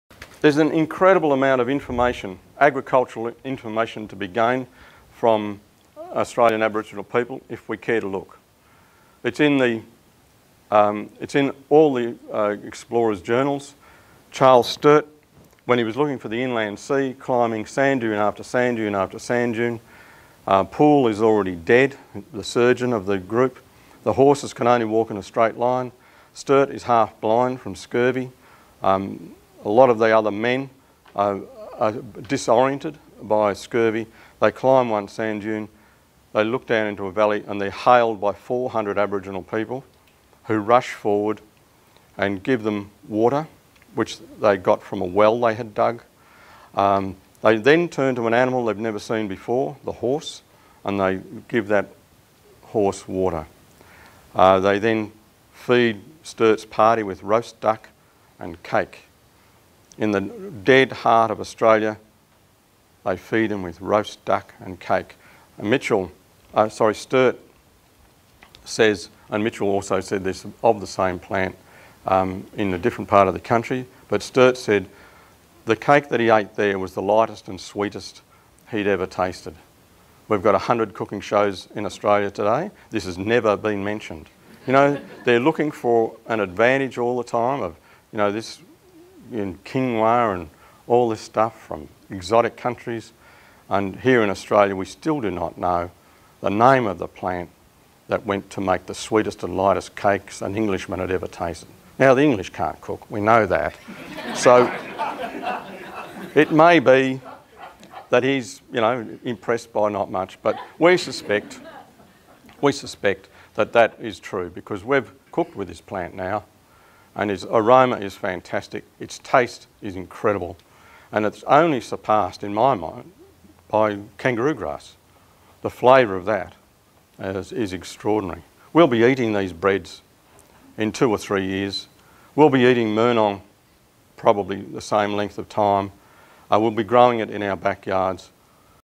Wonderful Stories From the Early Australian Explorer | FarmingSecrets.com
Bruce Pascoe says that there's an incredible amount of information, agricultural information, to be gained from Australian aboriginal people if we care to look. It's in all the explorers' journals. Watch and listen as he shares about Charles Sturt.